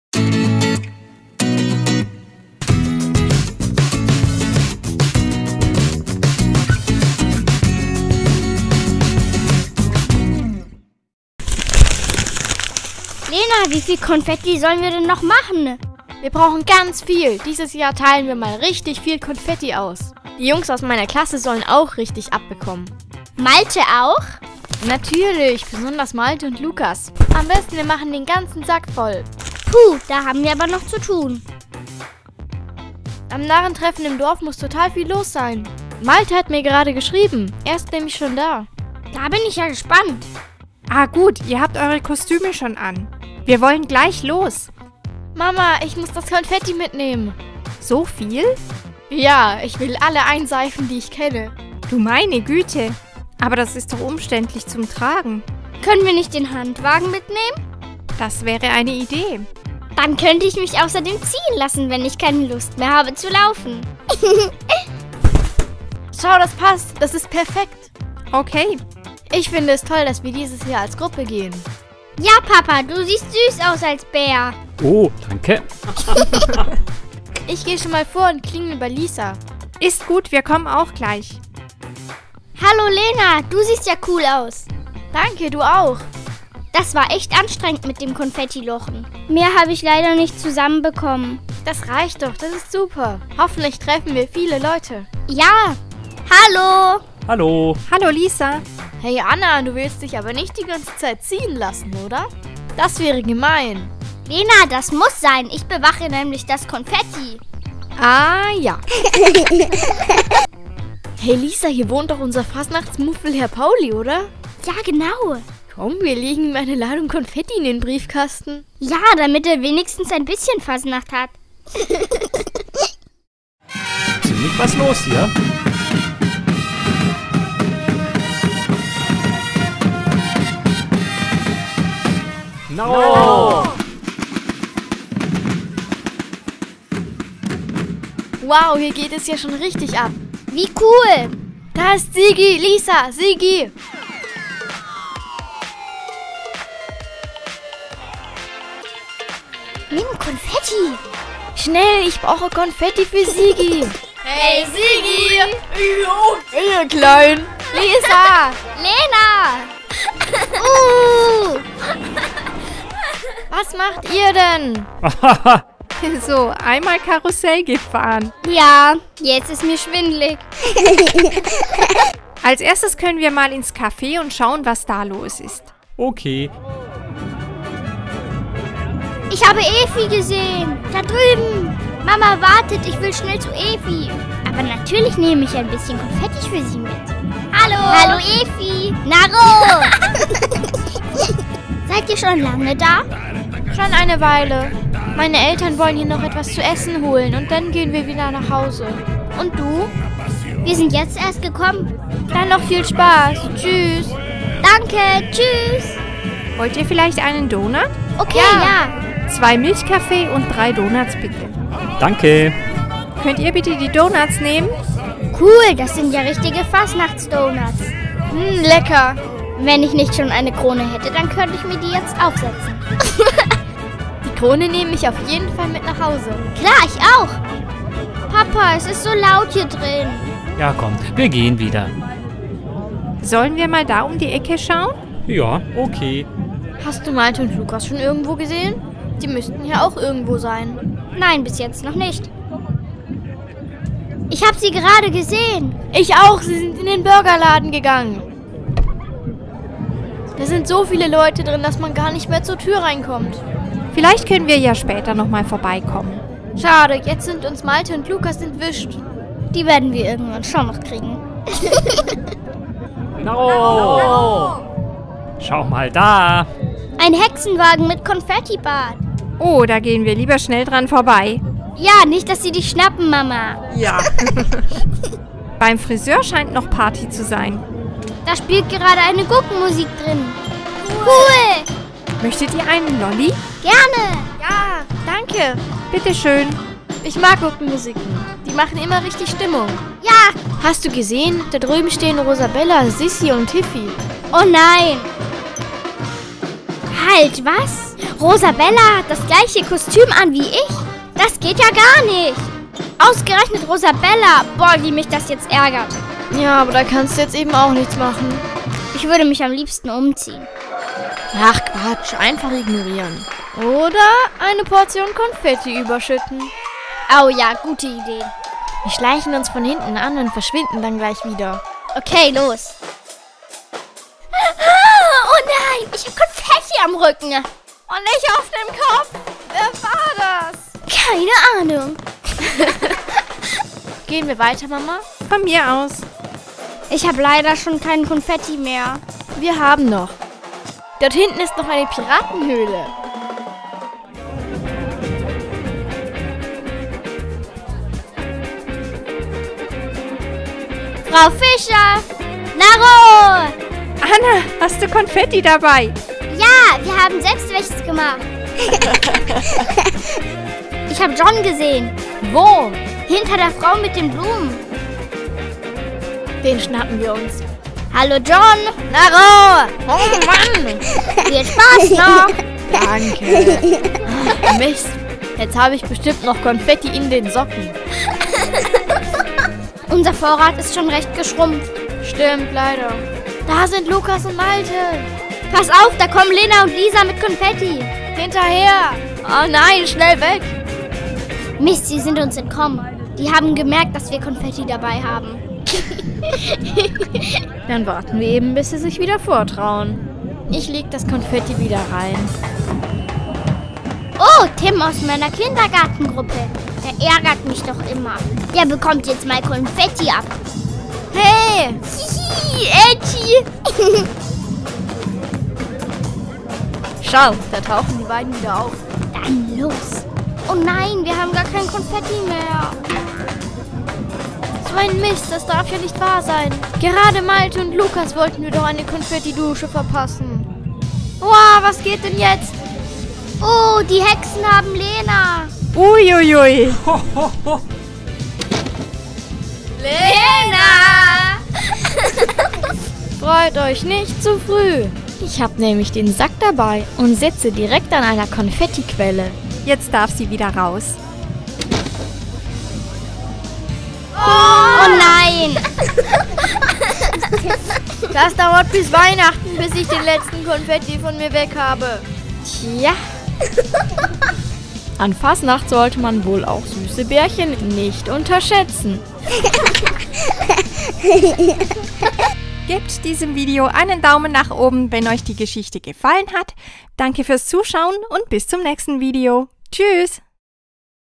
Playmobil Familie Hauser am Narrentreffen 2024 - Karneval Fasching Fastnacht
PLAYMOBIL Familie Hauser: Es wird Fastnacht gefeiert bei Familie Hauser. Kostüme anziehen und los geht es zum Narrentreffen! Viel Spaß!